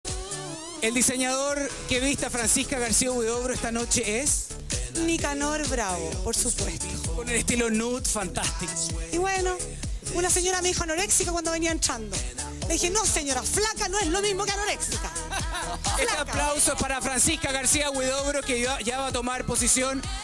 Francisca Garcia Huidobro pies zapatos Gala Viña 2011-02-18.mpg
Francisca Garcia Huidobro pies zapatos Gala Viña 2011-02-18.
Unos de los pocos acercamientos de los zapatos y pies de fran.